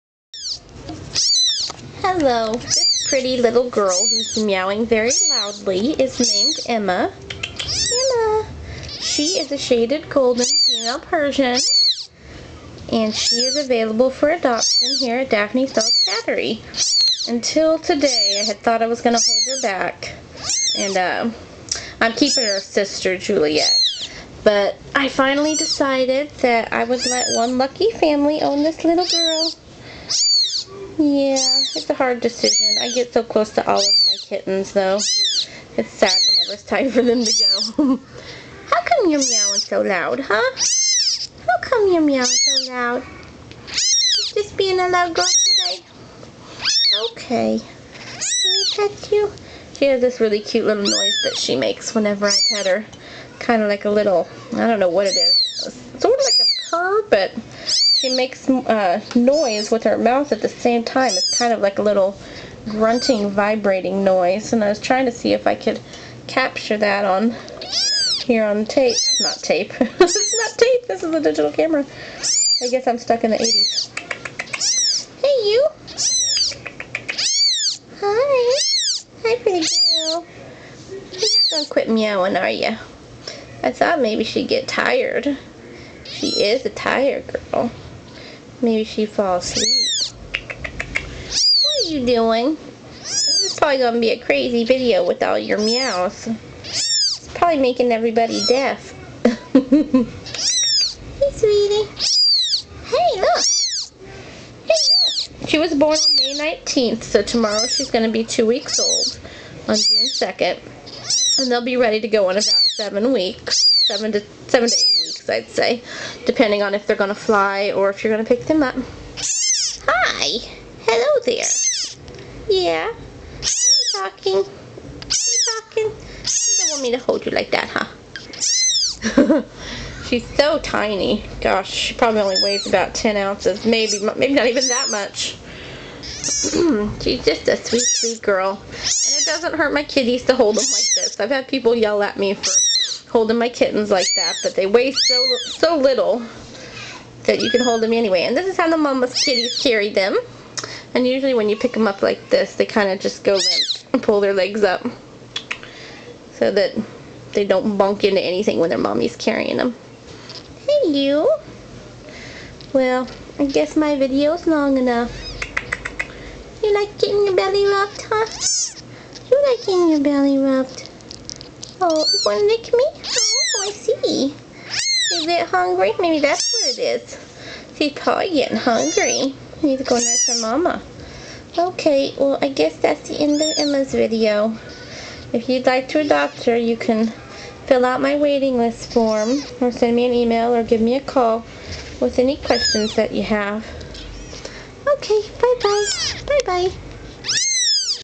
Emma - Shaded Golden Persian Kitten for Sale from Daphne's Dolls Cattery
This is little Emma.  She is almost 2 weeks old!  Emma is currently available for adoption at Daphne's Dolls Cattery.  She is a shaded golden Persian kitten and she was born on May 19, 2010.  if you would like to adopt Emma or one of our other persian kittens!